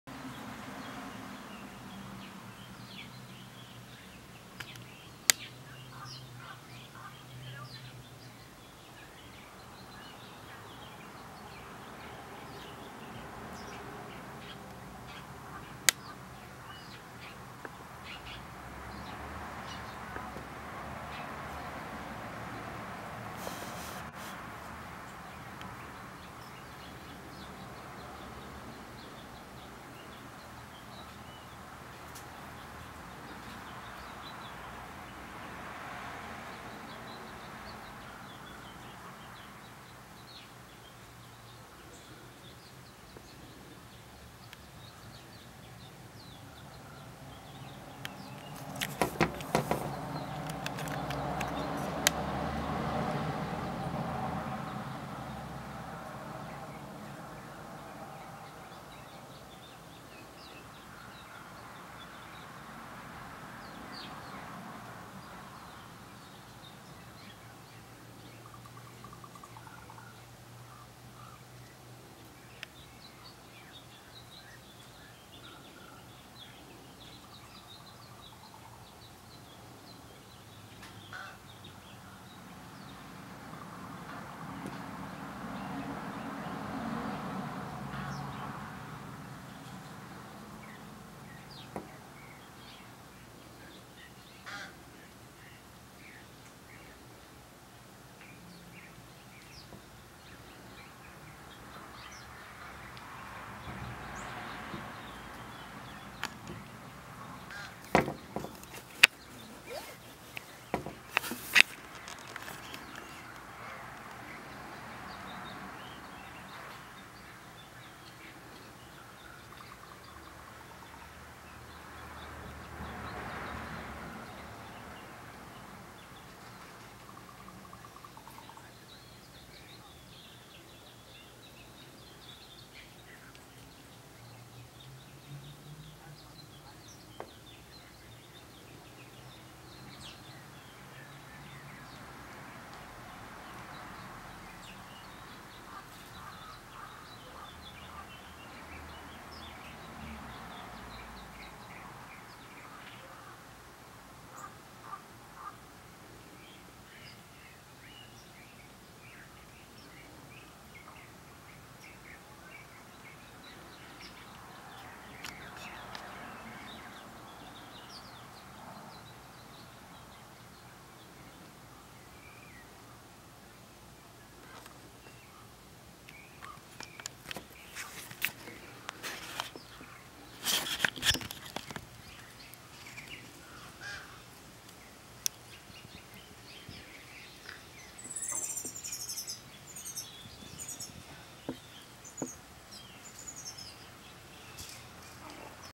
Backyard Bird Show
Playing around with Video Canon T2i on monopod some Back yard Bird Feeder and Bath action